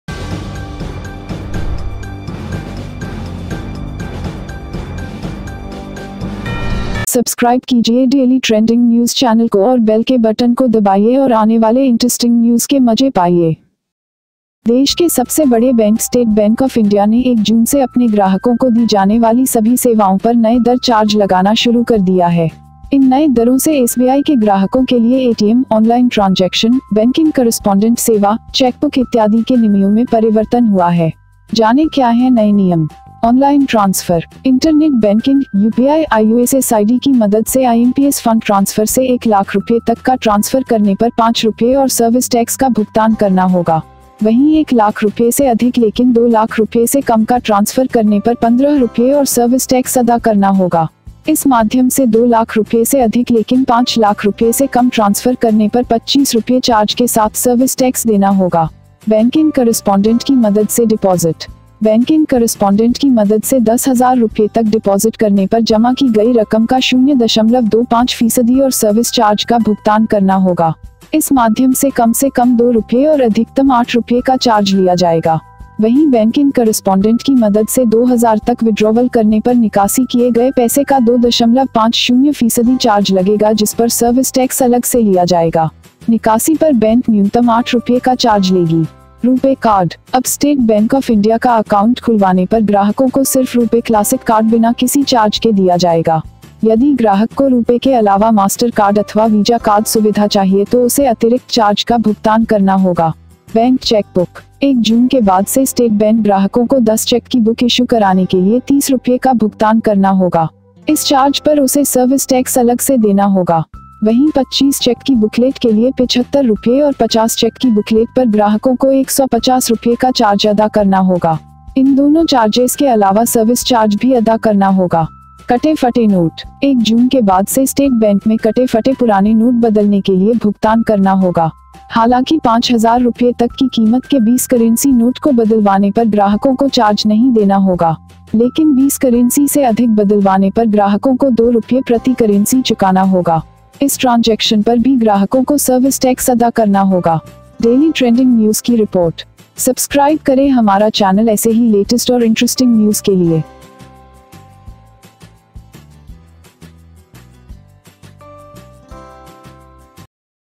SBI बैंक में अगर है आपका खाता, तो आज से लागू हुए ये नये नियम ।।
Nationalised banks such as State Bank of India (SBI), Bank of Maharashtra and Canara Bank have hiked charges for several services or facilities provided to customers. These include increase in charges for chequebook issuance, signature verification, cash handling charges, withdrawals beyond stipulated numbers and card issuance charges. Interestingly, Bank of Maharashtra, in a communication issued to its officers and clerks, has asked them to levy these charges manually on a daily basis as the system is not doing the job!